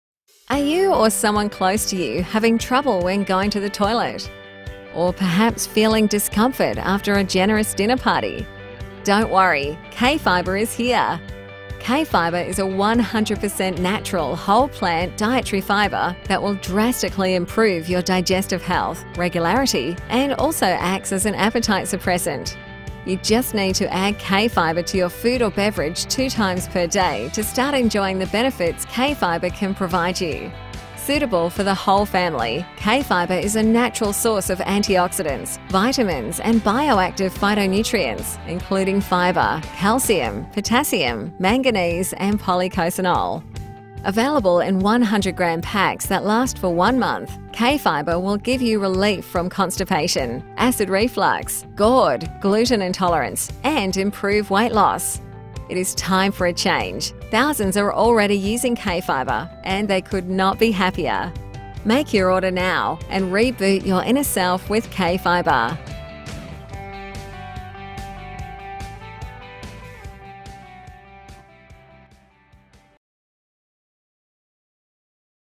Kfibre Digestive Health
Kfibre has been an inclusion in several successful trials in areas of gut health, constipation, gut inflammation, acid reflux and weight loss studies.